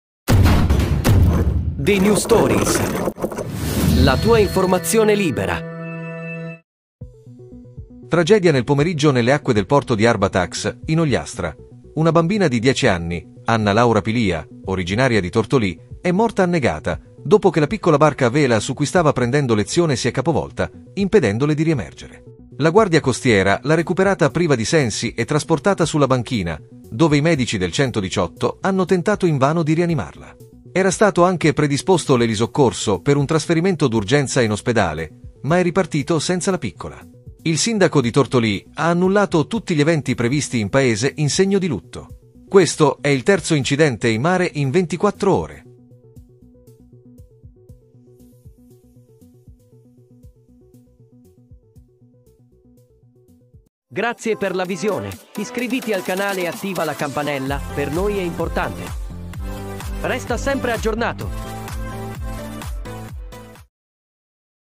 MUORE ANNEGATA A 10 ANNI, Anna Laura è caduta in acqua durante la lezione di vela in Sardegna.
Tragedia nel pomeriggio nelle acque del porto di Arbatax, in Ogliastra: una bambina di 10 anni, Anna Laura Pilia, originaria di Tortolì, è morta annegata dopo che la piccola barca a vela su cui stava prendendo lezione si è capovolta, impedendole di riemergere. La Guardia Costiera l'ha recuperata priva di sensi e trasportata sulla banchina, dove i medici del 118 hanno tentato invano di rianimarla. Era stato anche predisposto l'elisoccorso per un trasferimento d'urgenza in ospedale, ma è ripartito senza la piccola. Il sindaco di Tortolì ha annullato tutti gli eventi previsti in paese in segno di lutto. Questo è il terzo incidente in mare in 24 ore